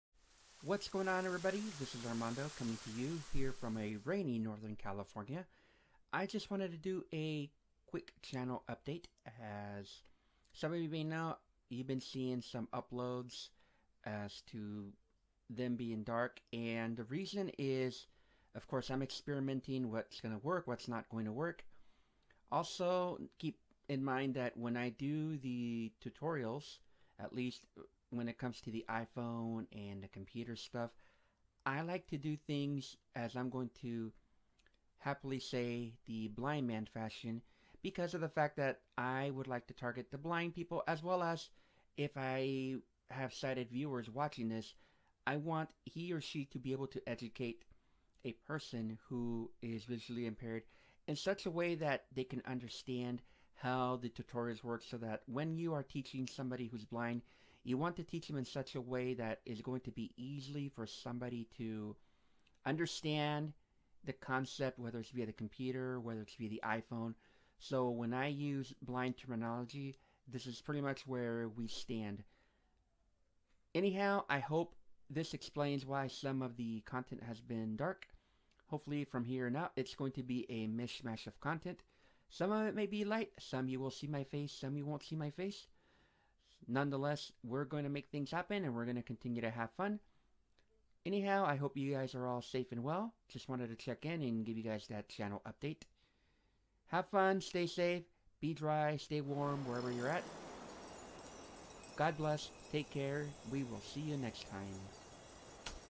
YouTube Channel Update, and why The dark Content
Greetings, this will hopefully explain why some of my content has been on the dark side while others has been with a sighted-like feel.